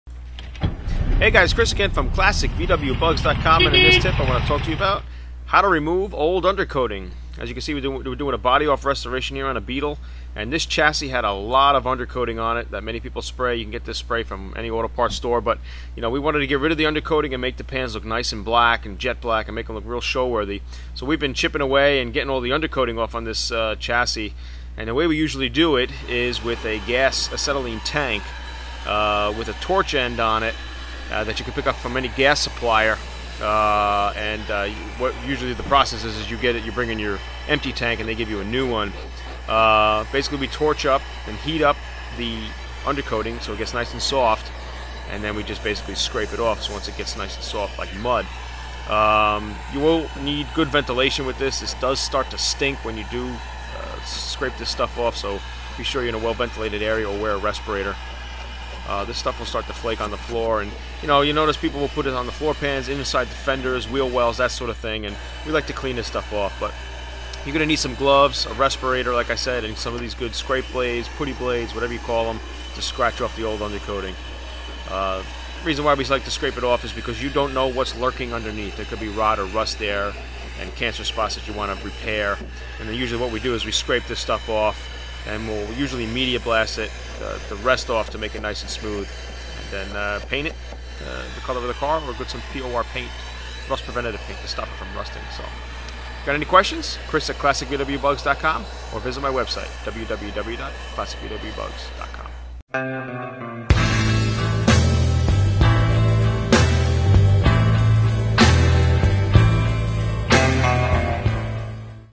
Classic VW BuGs How to Remove Old Undercoating Beetle Ghia BuS
In this Short video, I discuss how to remove old undercoating from your Vintage VW